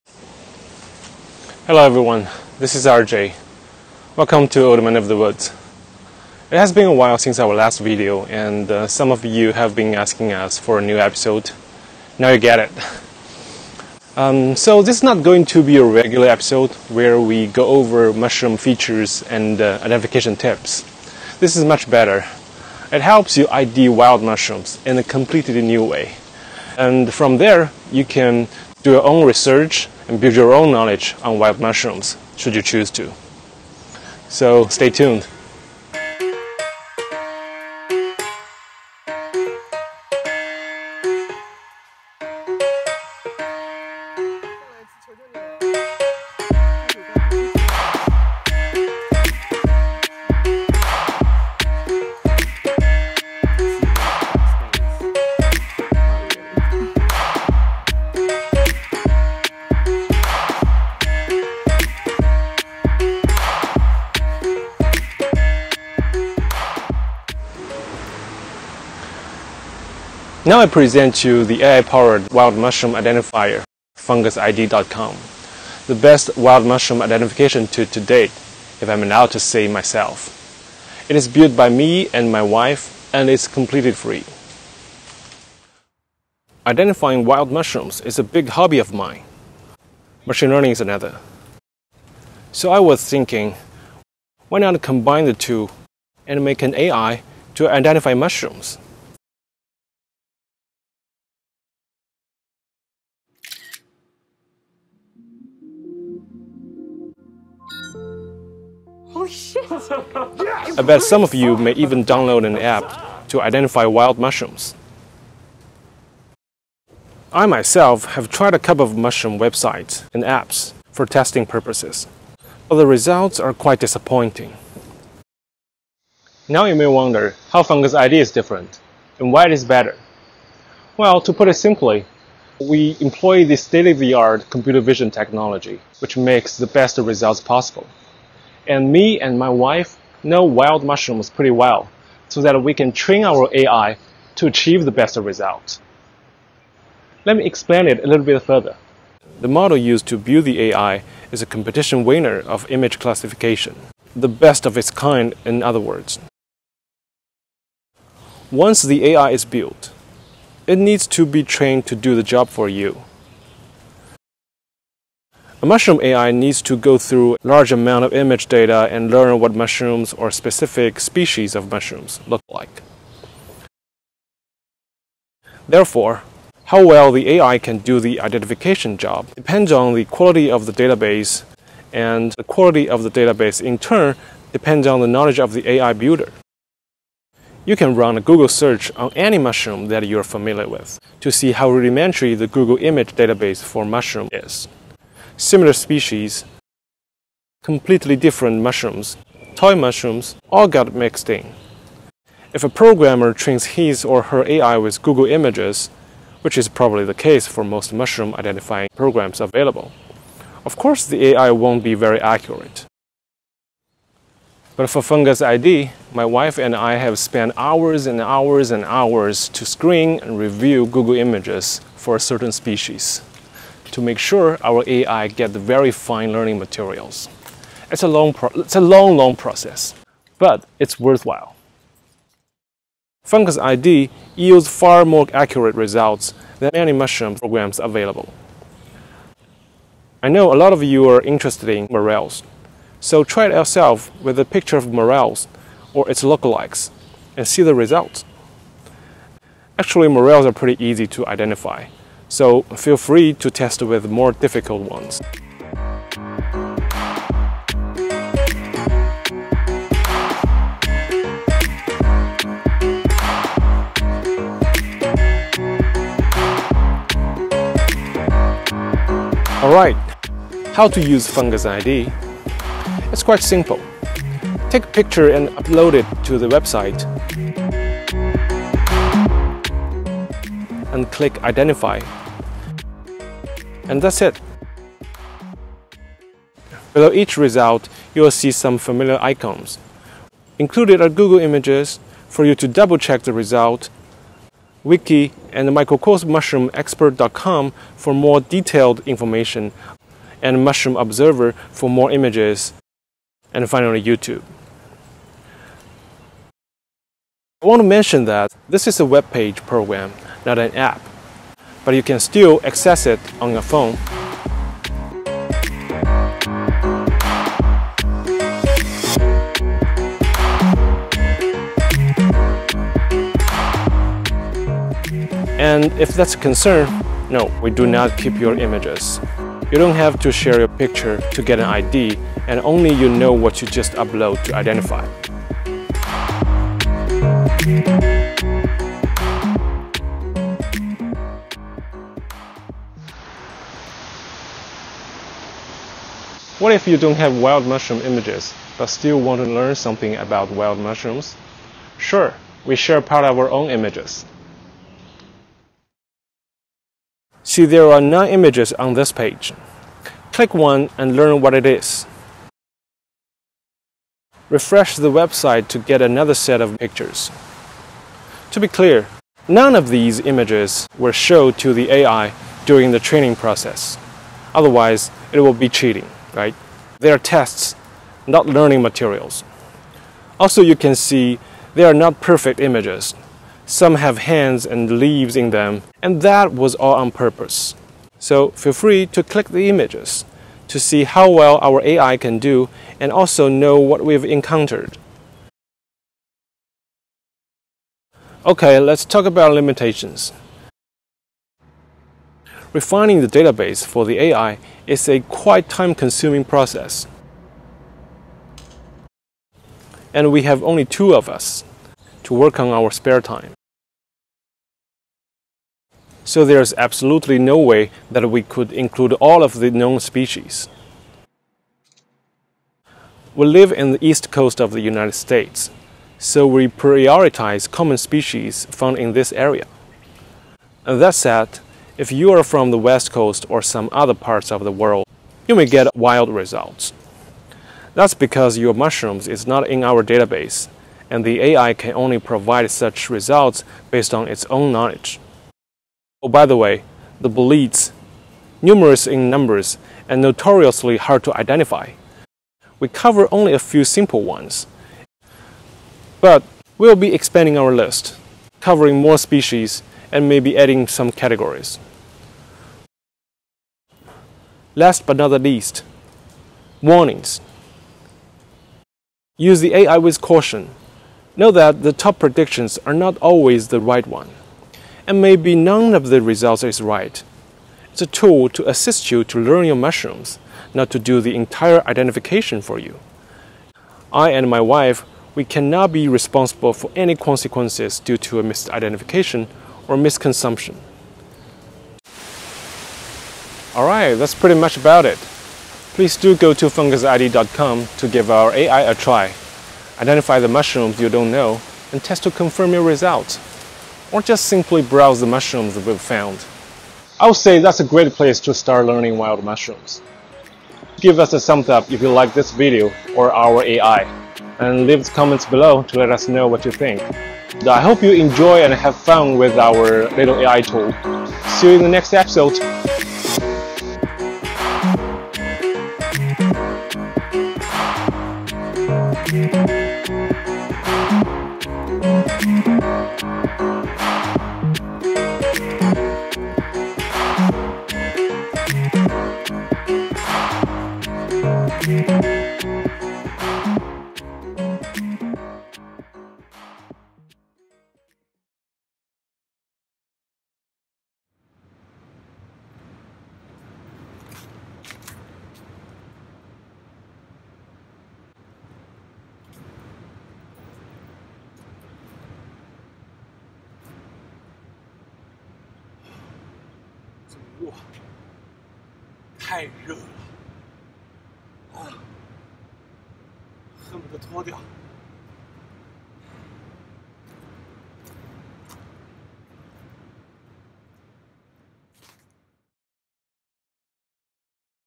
We Developed a FREE App to Help You Identify Mushrooms (Just Upload Your Photos!)
Identify mushrooms by photo. FREE fungus identification tool: why it's better & how to use

Be cautious &  always refer to multiple reliable sources before consuming any wild edibles!